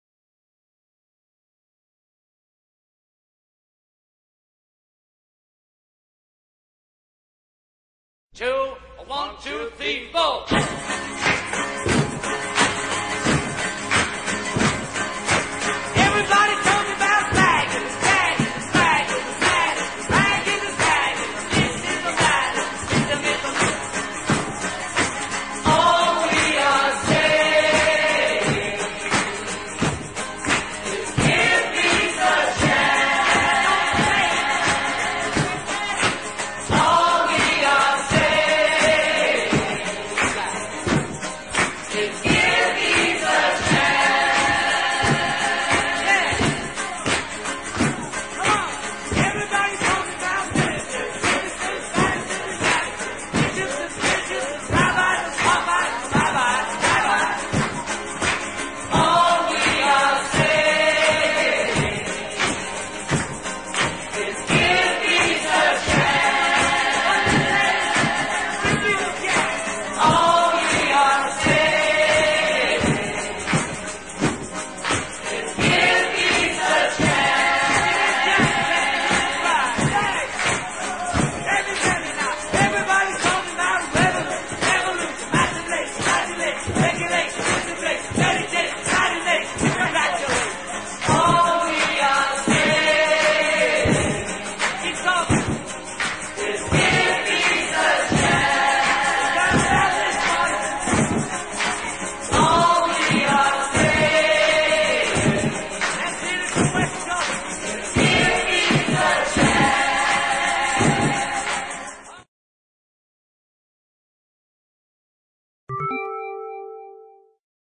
John And Yoko
John and Yoko montage honouring the new movie "The US vs John Lennon" and of course, John himself..  RIP